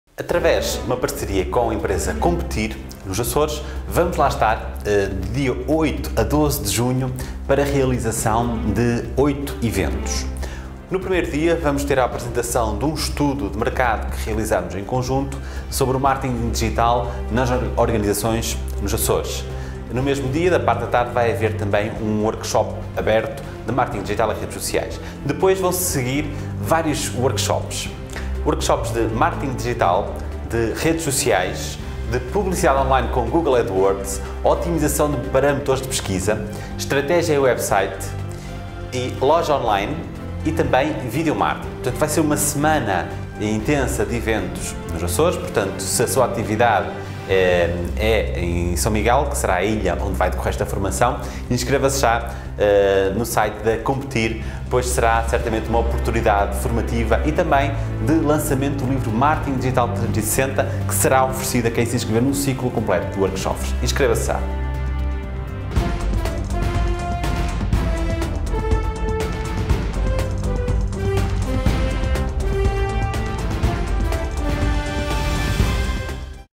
Semana Marketing Digital – Ponta Delgada | Açores
A Competir-Açores realiza entre os dias 8 e 12 de junho, na cidade de Ponta Delgada, um evento subordinado ao tema do Marketing Digital, com a participação de Vasco Marques, autor do livro Marketing Digital 360.

A iniciativa irá englobar a apresentação dos resultados do estudo “Marketing Digital: O Posicionamento das Organizações na Região Autónoma dos Açores” (que está a ser desenvolvido pela Competir-Açores em parceria com o Vasco Marques), bem como a realização de 8 workshops ao longo da semana e que serão dedicados a diferentes temas:  Redes Sociais, Publicidade no Google Adwords, Estratégia Digital e Website, Otimização para Motores de Pesquisa, Vídeo Marketing e Loja Online.